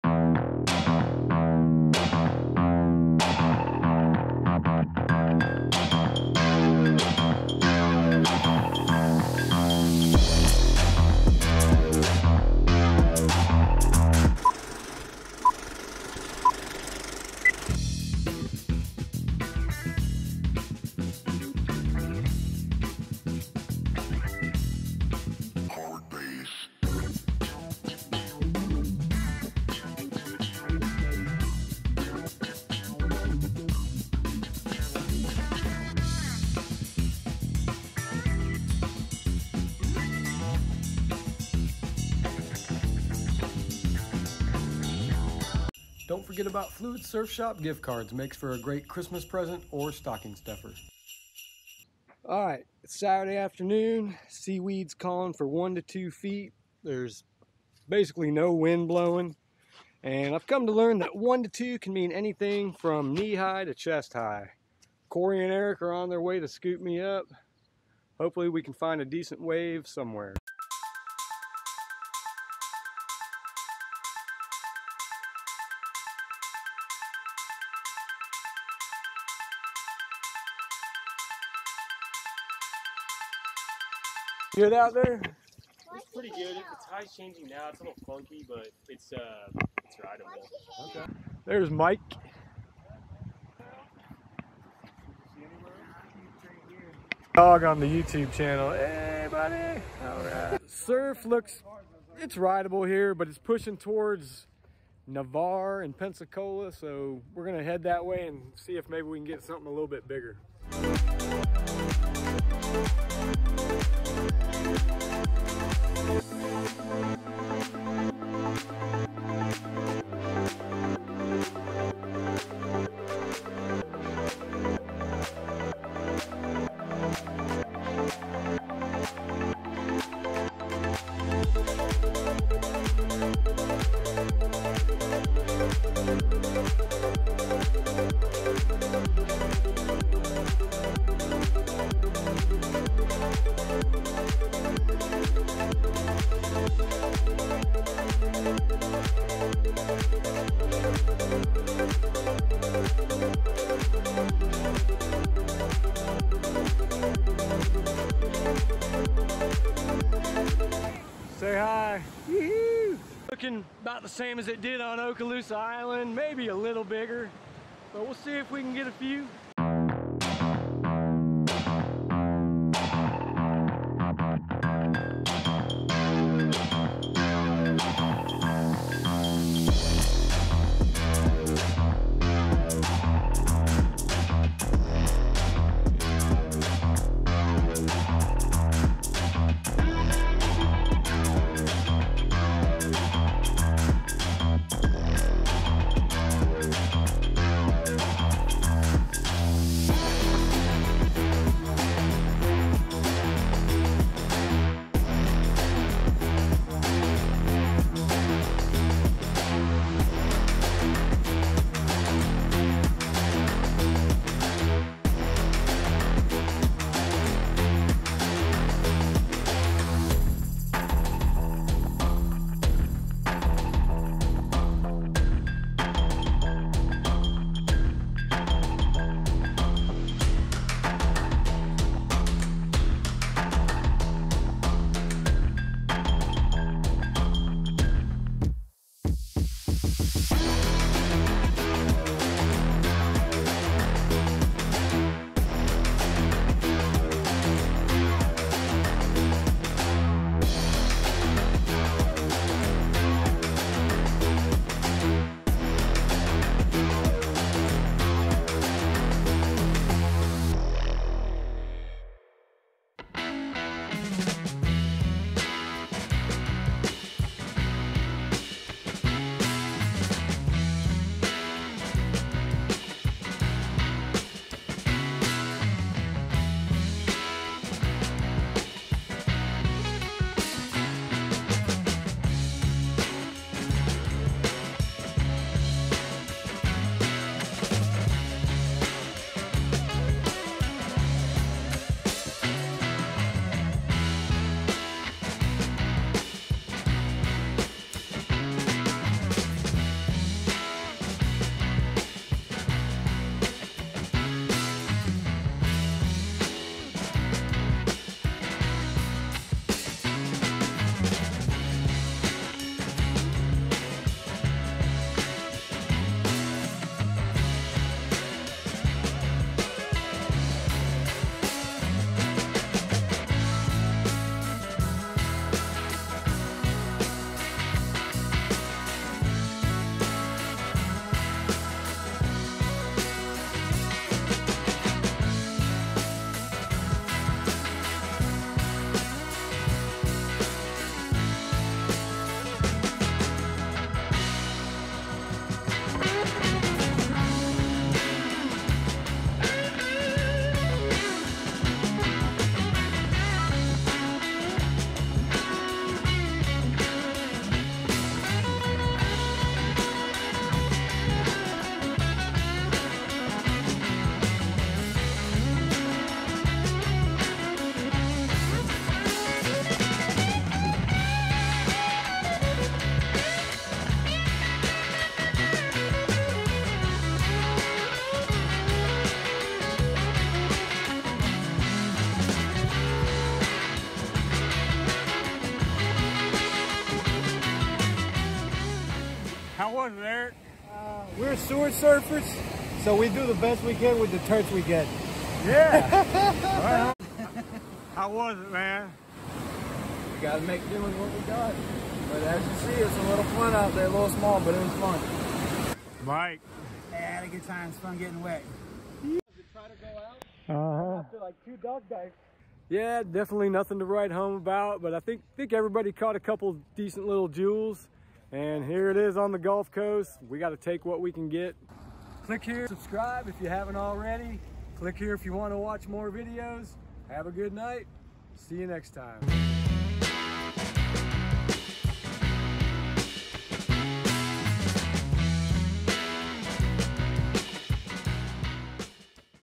Evening Glass in Navarre Beach Pier Surfing Gulf Coast Florida Santa Rosa County Emerald Coast Drone
Late afternoon trip over to Navarre Beach…